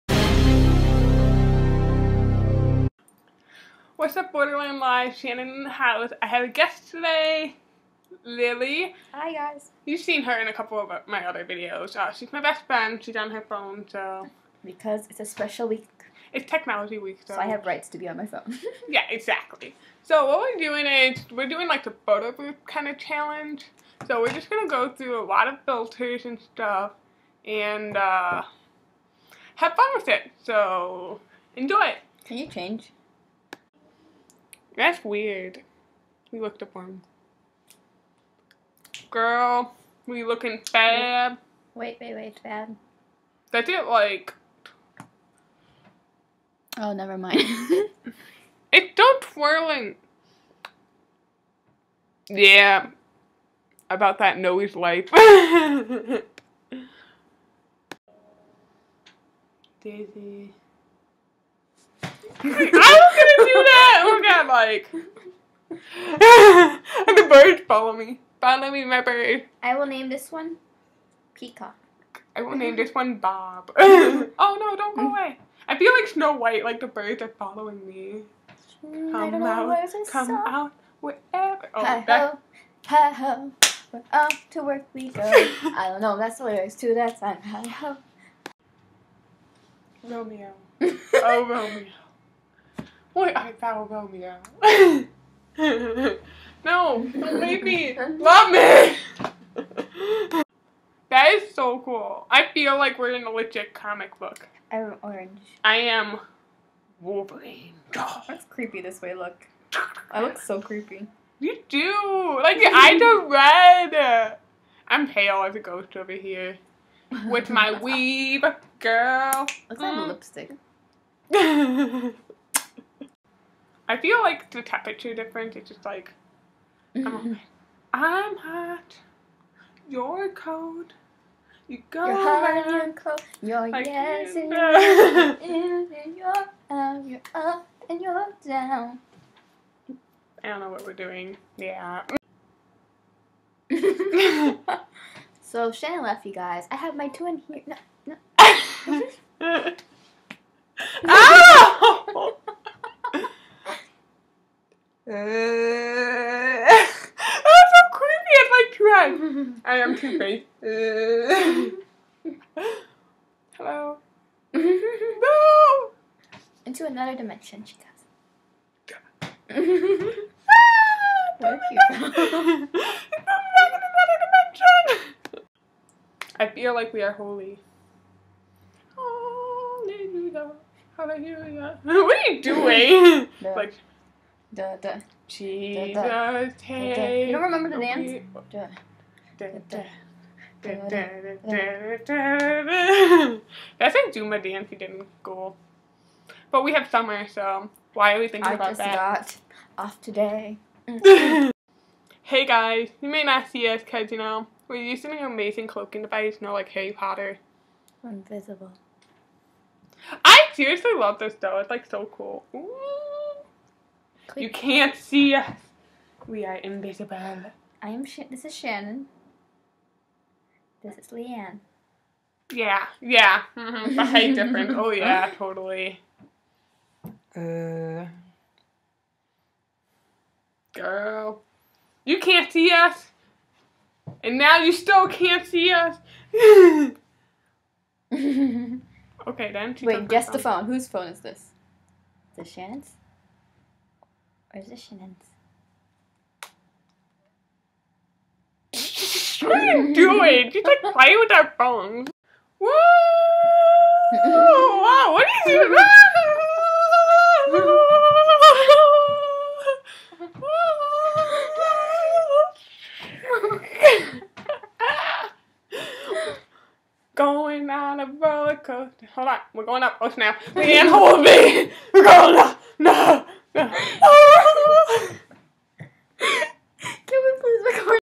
Tech Week | Shannon
It's technology week! Me and my best friend, Lianne do the Photobooth challenge!
:) (Apologies on not having a video last week.. I was having technical difficulties..)

..Oh, I forgot to do the ending. PEACE.. I LOVE YOU!!! BYE. :)